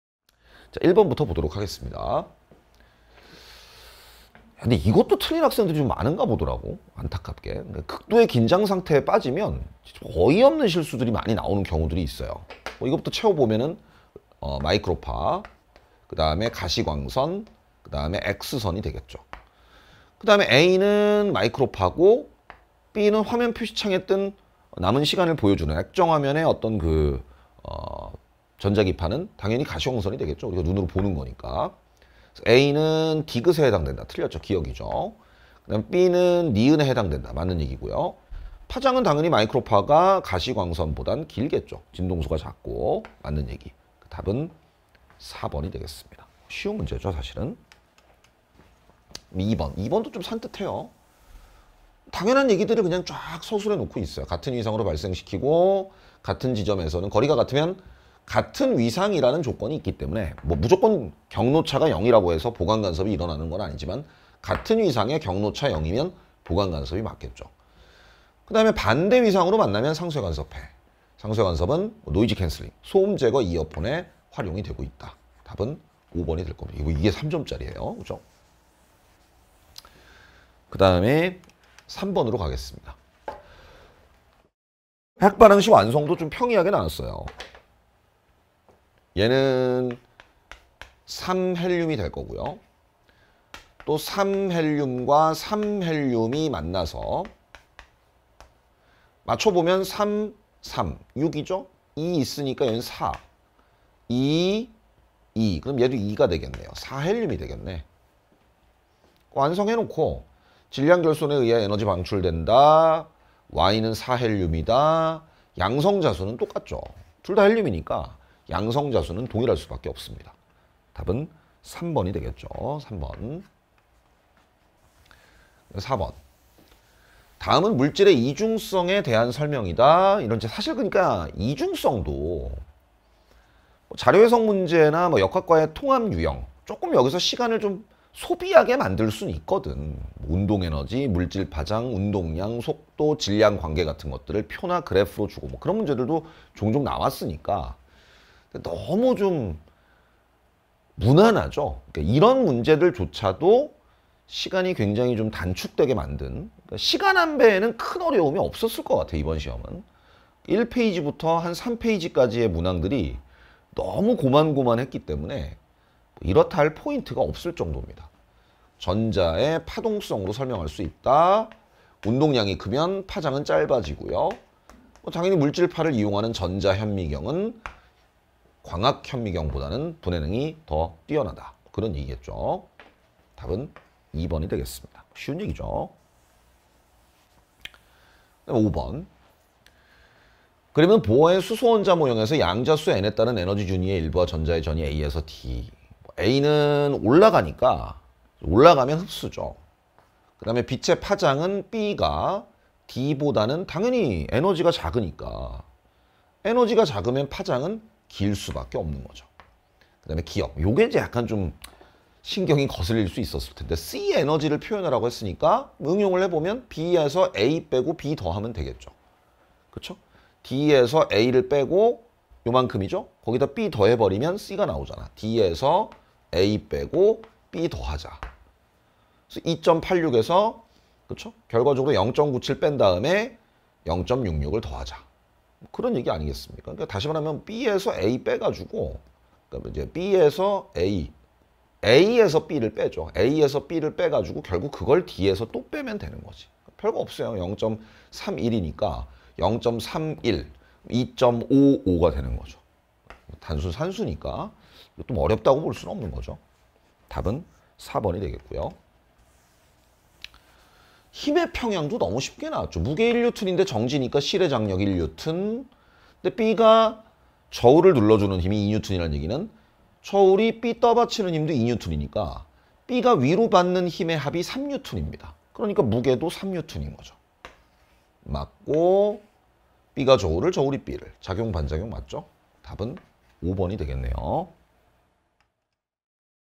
[메가스터디] 물리학 배기범쌤 - 2023학년도 고3 수능 물리학l 해설강의
물리학 배기범 선생님의 2023학년도 수능 물리학 해설강의 영상입니다.

- 물리학 배기범 선생님을 더 알고싶다면?